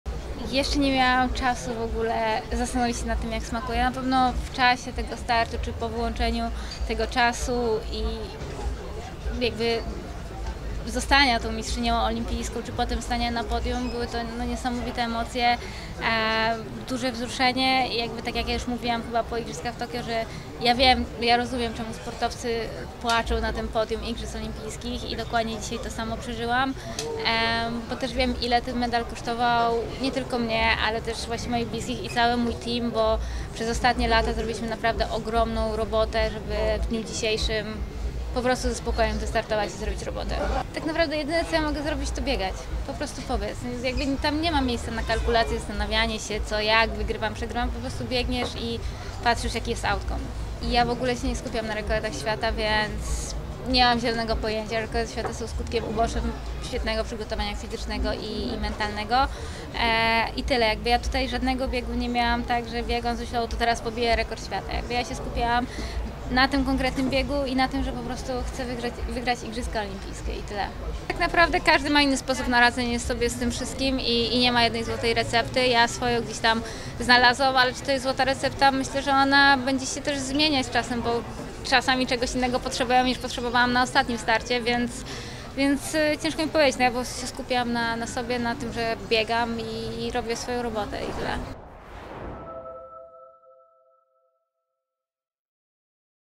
Mirosław zdeklasowała konkurencję. Złota medalistka: Nie ma miejsca na kalkulacje
Aleksandra Mirosław zdobyła pierwsze złoto dla Polski na igrzyskach olimpijskich w Paryżu! Polka podczas imprezy nie chciała rozmawiać z mediami, wolała skupić się na występach. Po zdobyciu medalu Mirosław w końcu opowiedziała nam o swoich odczuciach.